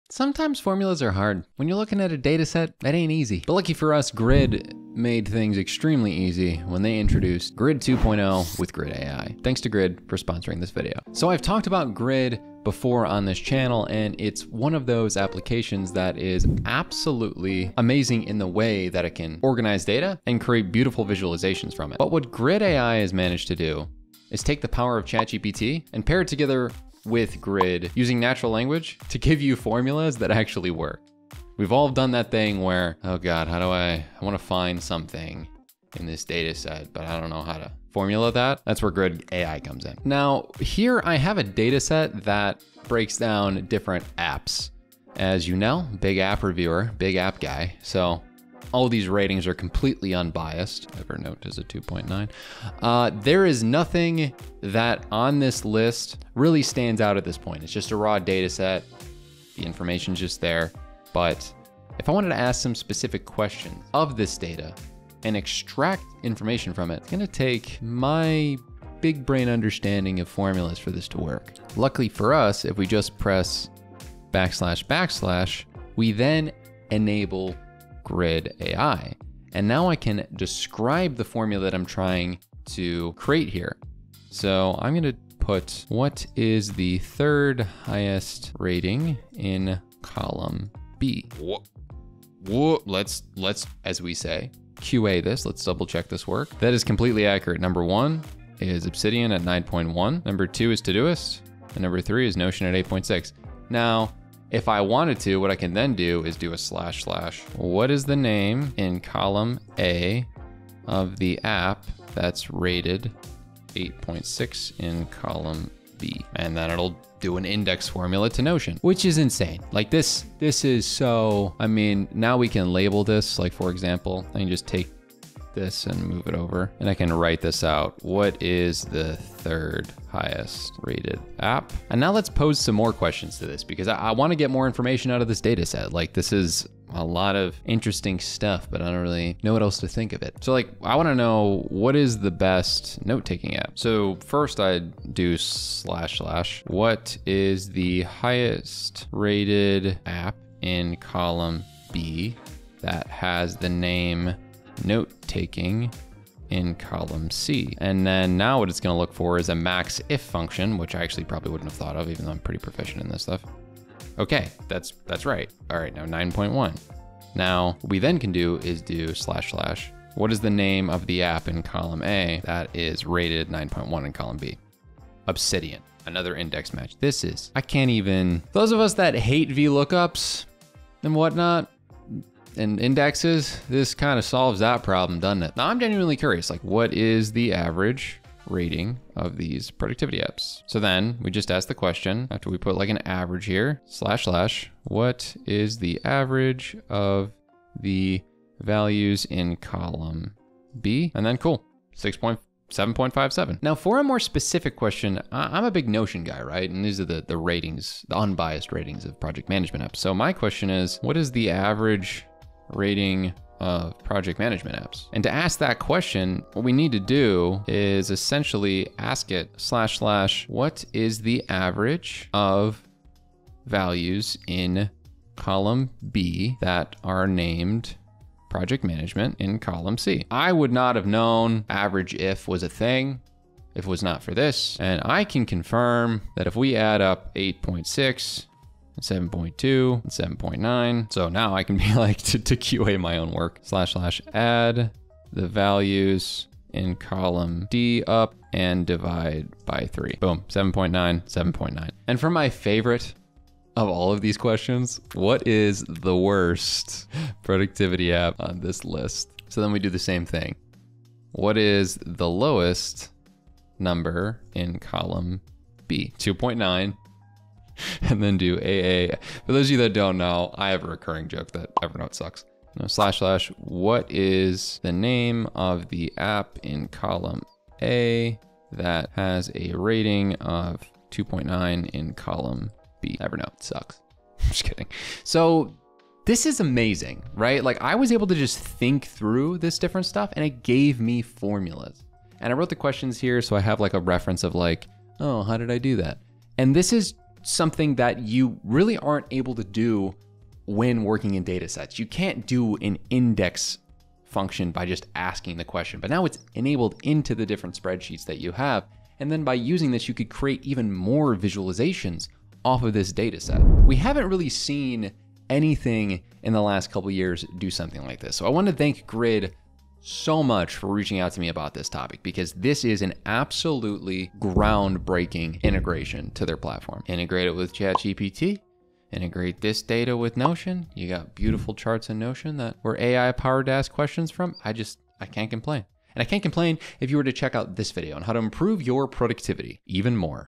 Why Grid AI is the BEST AI Assistant
Discover why GRID is the ultimate AI assistant for anyone who works with numbers. With its intuitive interface and powerful AI algorithms, GRID can analyze even the largest data sets and turn them into stunning visuals. From data import and organization to advanced analytics and data visualization, see why professionals are choosing GRID over traditional spreadsheet software. Watch this video to discover how GRID - A magical surface for numbers - can transform the way you work with data.

⏰ TIMESTAMPS:
0:00 - Sometimes Formulas Are Hard
0:05 - Lucky For Us Grid Made Things Easier
0:25 - Grid AI
0:41 - Exploring Grid AI